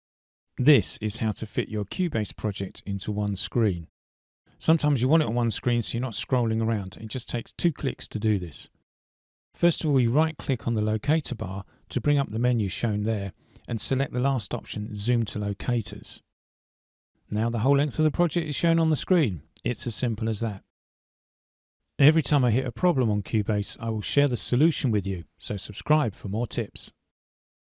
Cubase. How to fit a Cubase Project into the Screen.
Cubase DAW Issues solved. Fitting the whole Cubase Project into the Screen

Just 2 clicks and you won’t have to scroll around to see your whole project.

Essentially right click on the locator bar and choose the last option “zoom to locators”